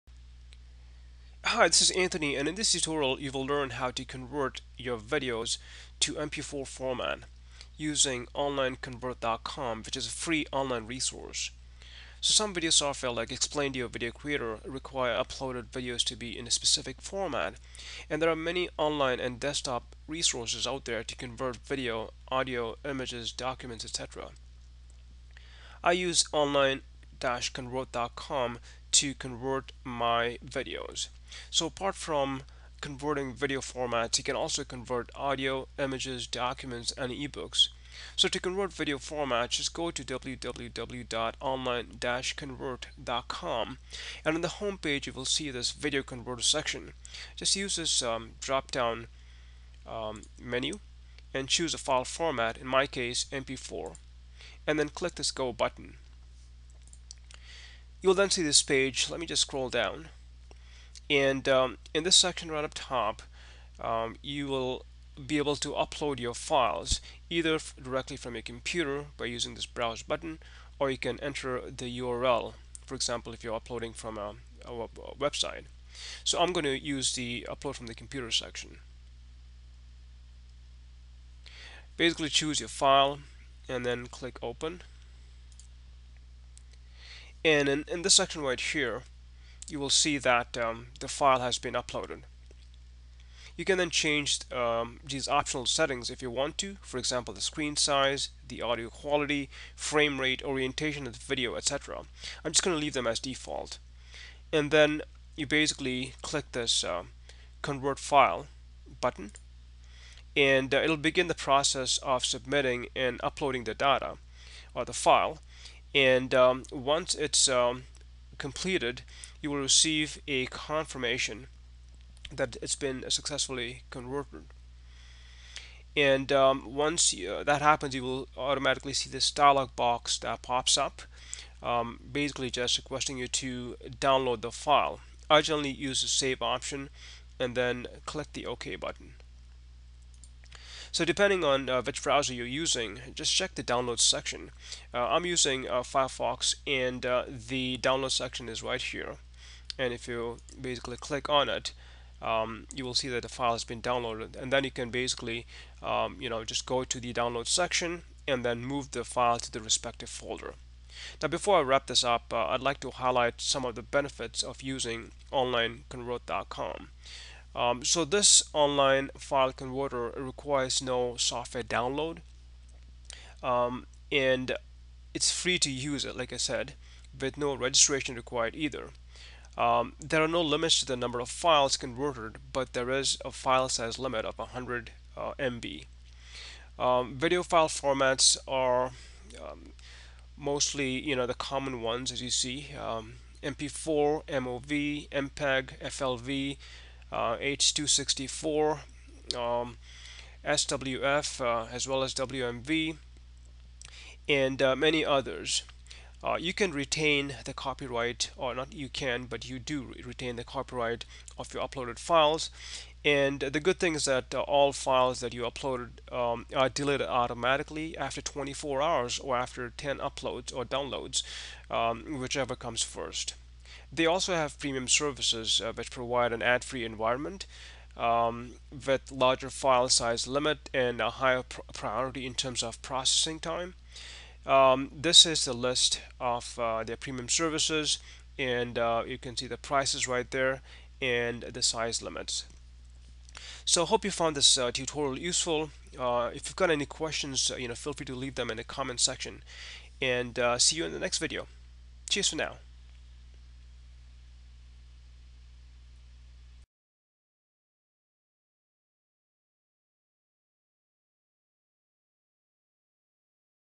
How to Convert Videos to MP4 Format
In this video tutorial you will learn how to convert videos to MP4 format using Online-Convert.com. Some applications require that your videos be encoded in a specific file format. Using Online-Convert you can convert videos to different file formats like MP4, MOV, MPEG, FLV, H264, SWF, WMV, and others.

You can convert files for free and upload them from your computer or by using a URL if you want to upload from a website. Before converting video file formats, you can also change various settings like screen size, audio quality, frame rate, orientation of the video, etc.

CREDITS:
- Online-Convert.com logo and screenshots are copyright protected used with permission.
- Explaindio logo and screenshots are copyright protected used with permission.